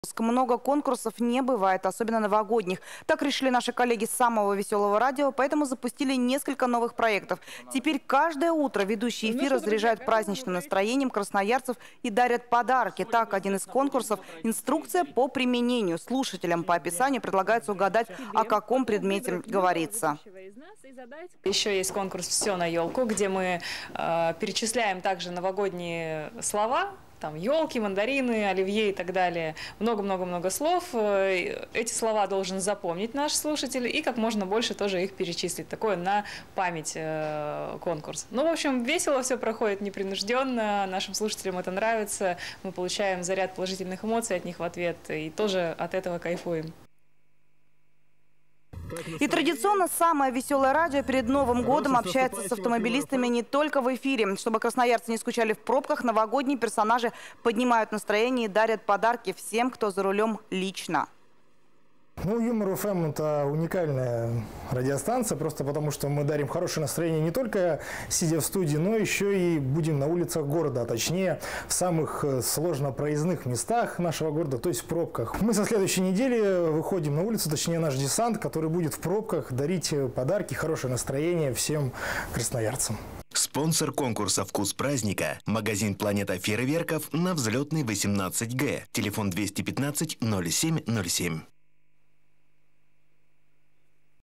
Самое веселое радио Красноярска запускает новогодние проекты
Каждое утро ведущие эфира заряжают праздничным настроением красноярцев и дарят подарки